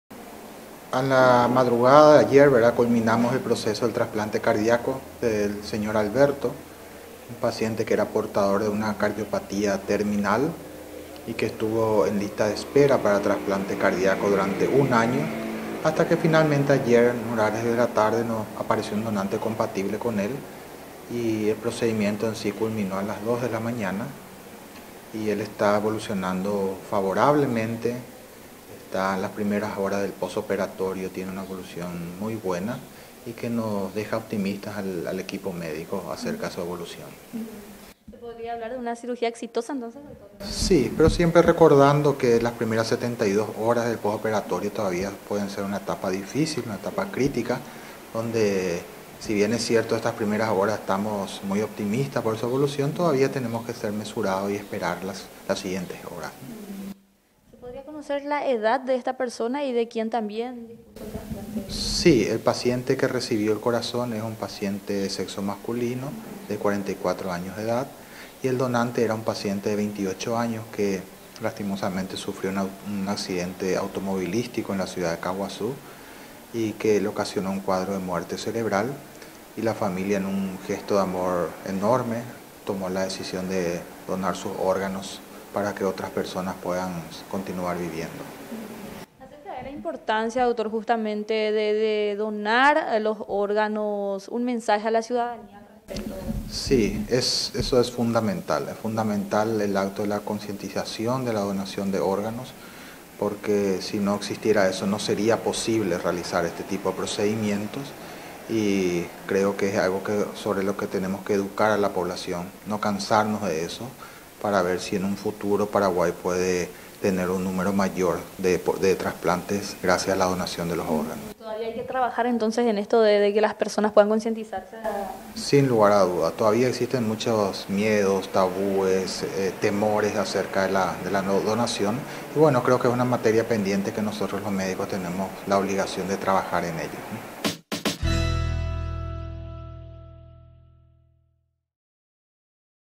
EXITOSO TRASPLANTE CARDIACO A PACIENTE, EN EL INSTITUTO NACIONAL DE CARDIOLOGÍA
Tras un año de espera un paciente de 43 años con cardiopatía dilatada en fase terminal fue beneficiado con un trasplante de corazón. Las 72 horas siguientes son fundamentales por lo que se hará el seguimiento médico de rutina. El Dr. Marcos Melgarejo, Director del Instituto Nacional de Cardiología, encabezó el procedimiento de trasplante de corazón a un paciente de 43 años cuya situación ya estaba en la parte final por el deterioro que significó la cardiopatía dilata que padecía. La cirugía fue exitosa y se prevé que en las siguientes 72 horas evolucione de favorablemente. Aquí la nota con el Doctor Melgarejo.